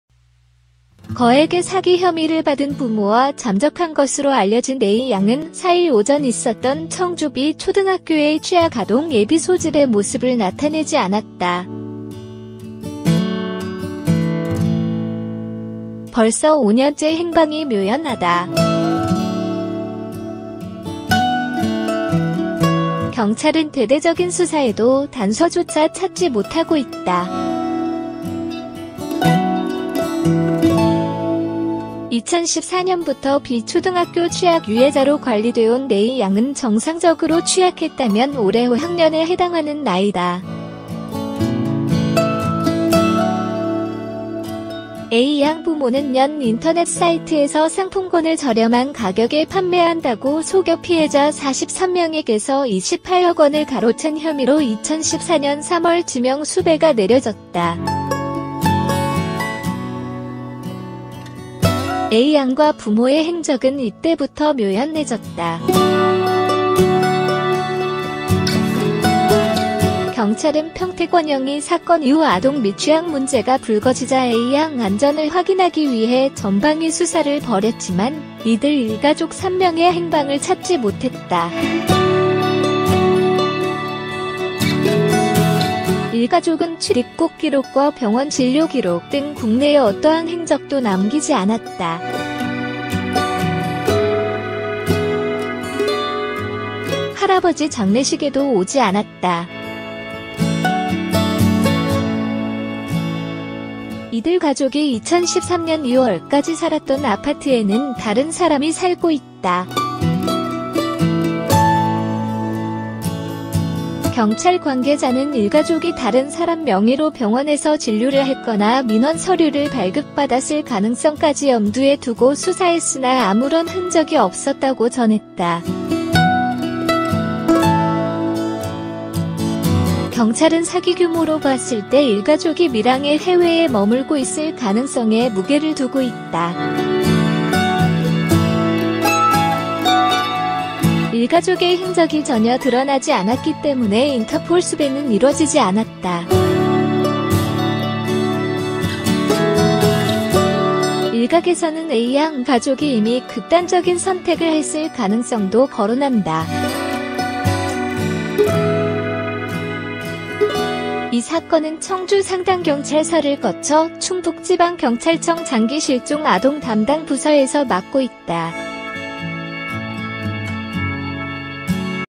5년째 행방불명인 11살 소녀의 부모를 지명수배합니다
5년째 행방불명인 11살 소녀의 부모를 지명수배합니다